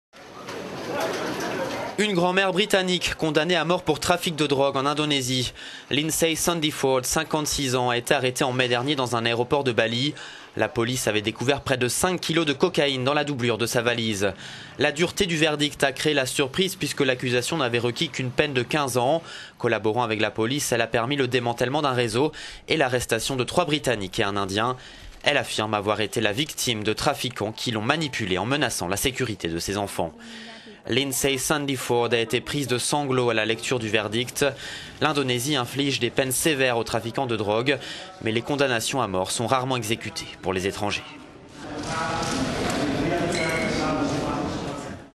Une britannique condamnée à mort en Indonésie
Une grand-mère britannique a été condamnée à mort pour trafic de drogue en Indonésie. Lindsay Sandiford, 56 ans, a été arrêtée en mai dernier dans un aéroport de Bali. La police avait découvert près de cinq kilos de cocaïne dans la doublure de sa valise.

La dureté du verdict a créé la surprise puisque l'accusation n'avait requis qu'une peine de 15 ans. Collaborant avec la police, elle a permis le démantèlement d'un réseau et l'arrestation de trois britanniques et un Indien. Elle affirme être la victime de trafiquants qui l'ont manipulé en menaçant la sécurité de ses enfants.

Lindsay Sandiford a été prise de sanglots à la lecture du verdict. L'Indonésie inflige des peines sévères aux trafiquants de drogue, mais les condamnations à mort sont rarement exécutés pour les étrangers.